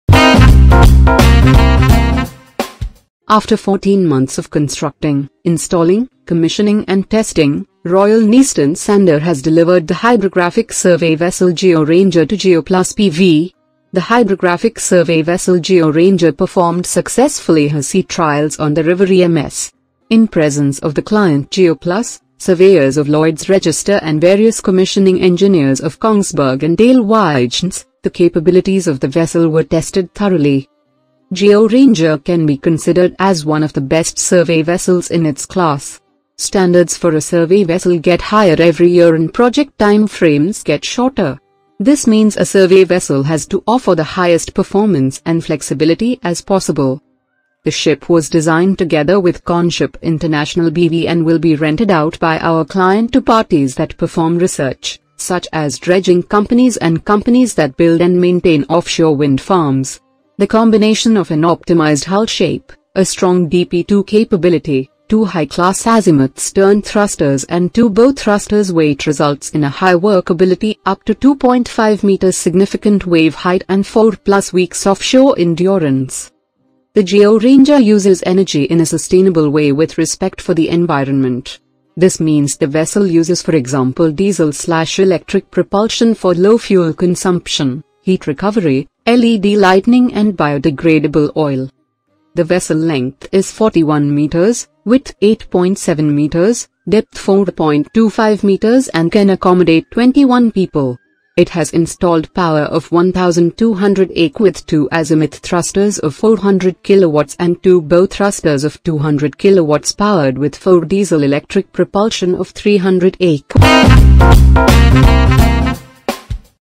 Royal Niestern Sander Delivers Hydrographic Survey Vessel Geo Ranger
After 14 months of constructing, installing, commissioning and testing, Royal Niestern Sander has delivered the hydrographic survey vessel Geo Ranger to Geo Plus BV. The hydrographic survey vessel Geo Ranger performed successfully her sea trials on the river Ems. In presence of the client Geo Plus, surveyors of Lloyd’s Register and various commissioning engineers of Kongsberg and Alewijnse, the capabilities of the vessel were tested thoroughly.

Geo Ranger can be considered as one of the best survey vessels in its class. Standards for a survey vessel get higher every year and project timeframes get shorter. This means a survey vessel has to offer the highest performance and flexibility as possible.

The ship was designed together with Conoship International BV and will be rented out by our client to parties that perform research, such as dredging companies and companies that build and maintain offshore wind farms. The combination of an optimized hull shape, a strong DP2 capability, two high class Azimuth stern thrusters and two bow thrusters Veth results in a high workability up to 2.5 meter significant wave height and 4+ weeks offshore endurance.

The Geo Ranger uses energy in a sustainable way with respect for the environment. This means the vessel uses for example diesel/electric propulsion for low fuel consumption, heat recovery, LED lightning and bio-degradable oil.

The vessel length is 41 metres, width 8.7 metres, depth 4.25 metres and can accommodate 21 people. It has installed power of 1200 ekW with 2 Azimuth thrusters of 400 kW & 2 bow thrusters of 200 kW powered with 4 Diesel-electric propulsion of 300 ekW